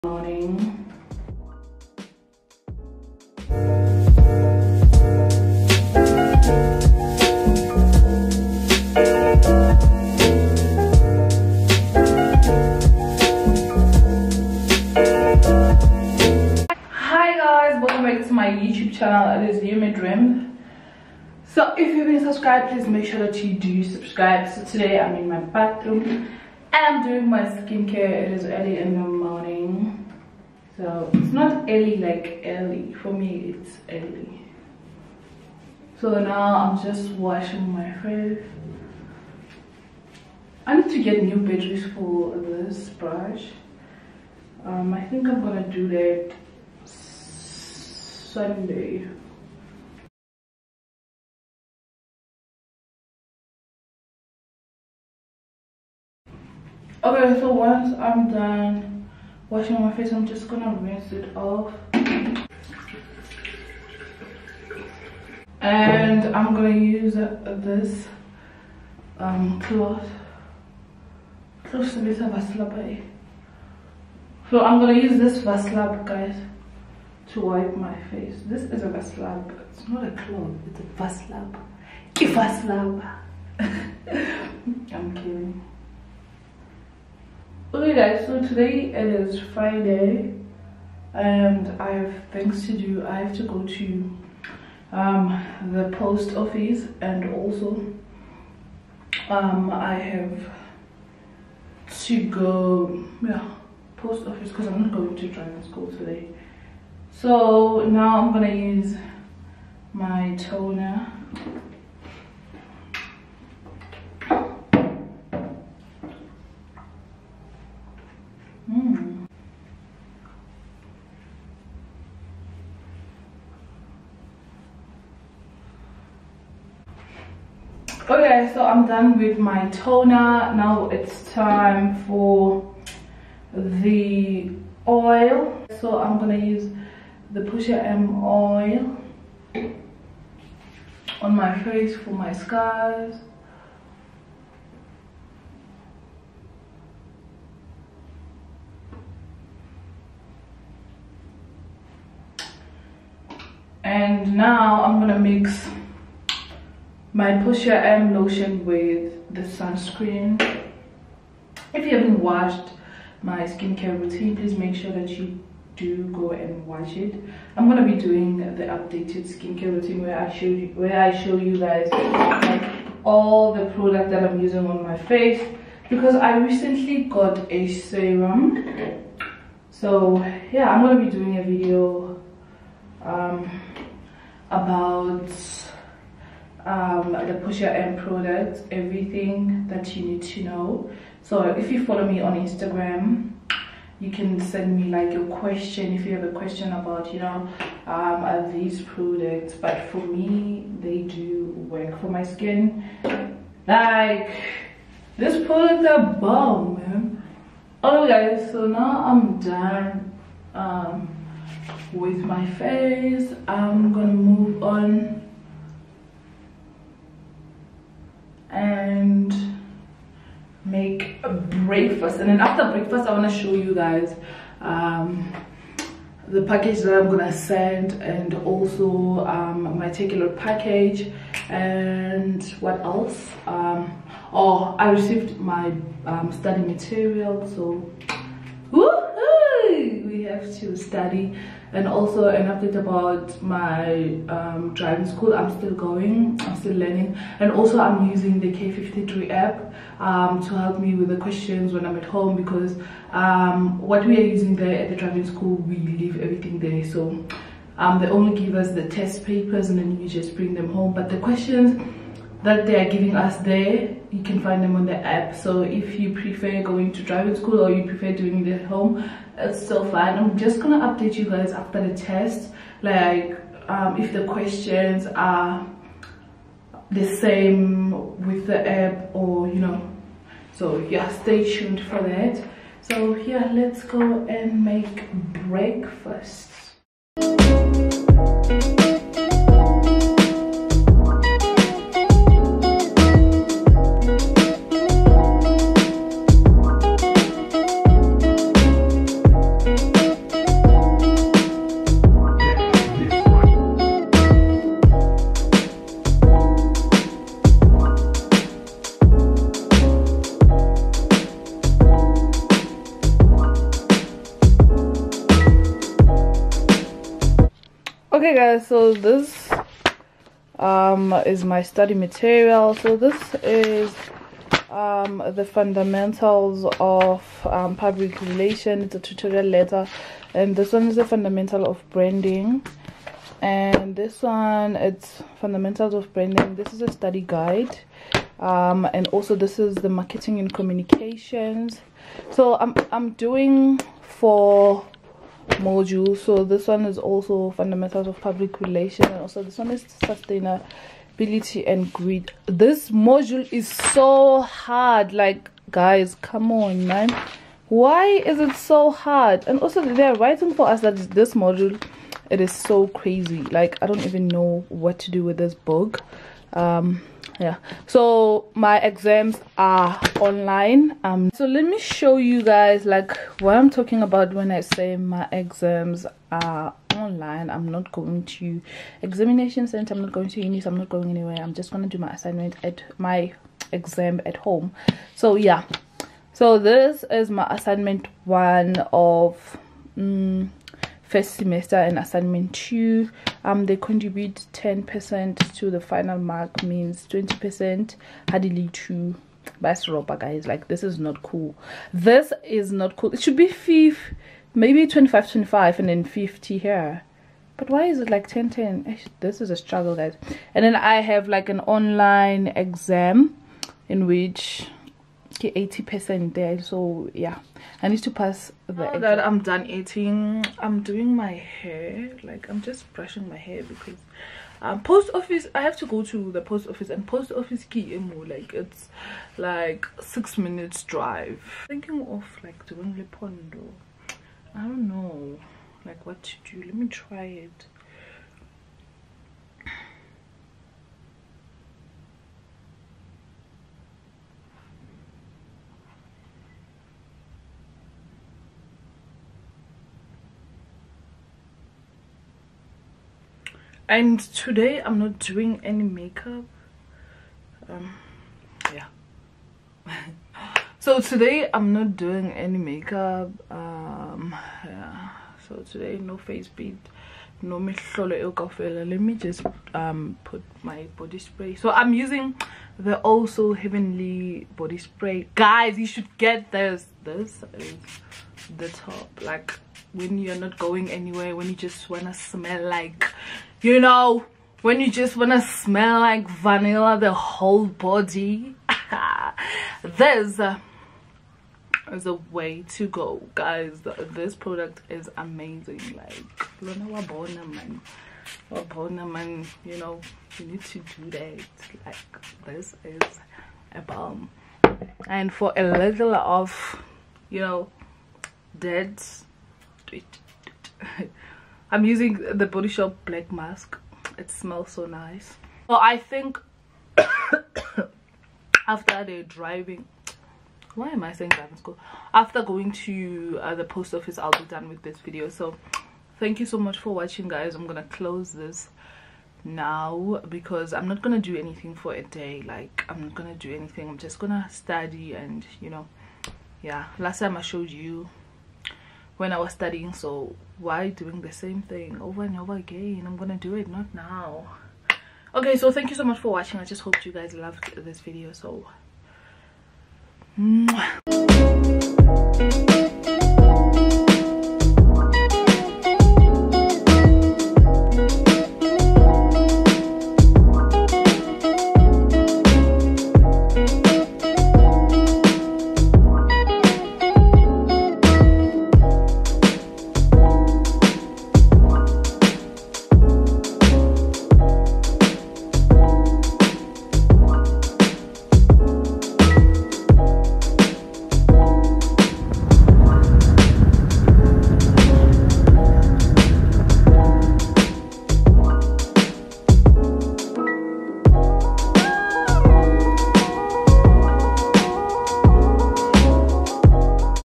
MORNING ROUTINE: I RECEIVED MY STUDY MATERIALS, UPDATED SKIN CARE USING PORTIA M + EDITING VIDEOS
Today I will be showing you my morning routine ❤️

FAQ :

Race : black
Birthday: 18 July
I’m from South Africa 🇿🇦

VIDEOS YOU WOULD LOVE TO SEE